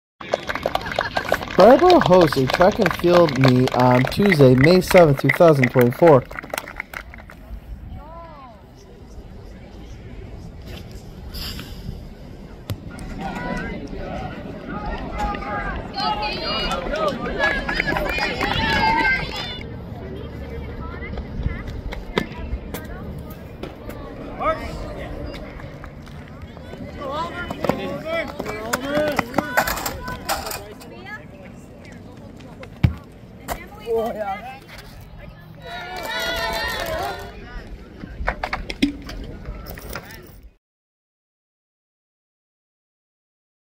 Track and field in Brattleboro
Brattleboro hosts a track and field meet on Tuesday, May 7, 2024.